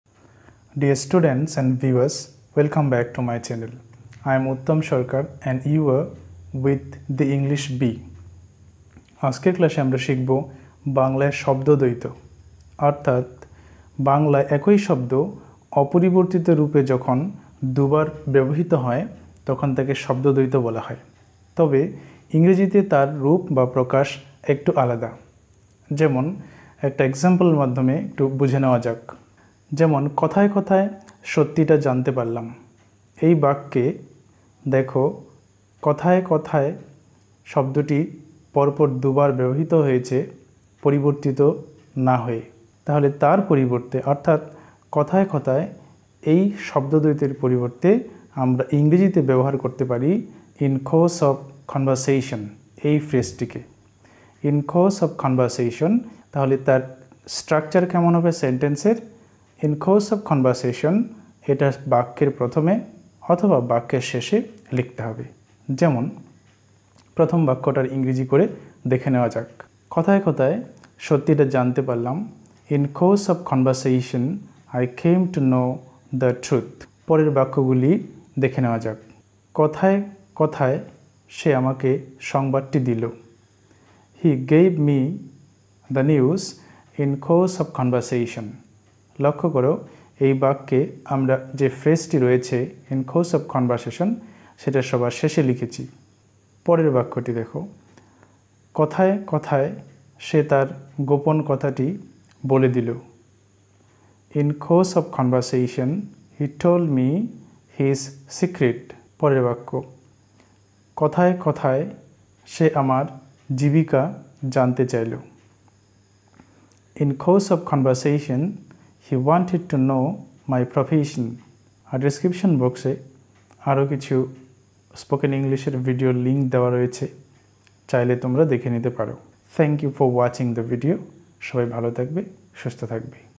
Spoken English| in course of conversation| how to use in course of conversation| কথায় কথায় ব্যবহার।
In this video we are going to learn how to use repetition of words 'in course of conversation.
we use this phrase in everyday English.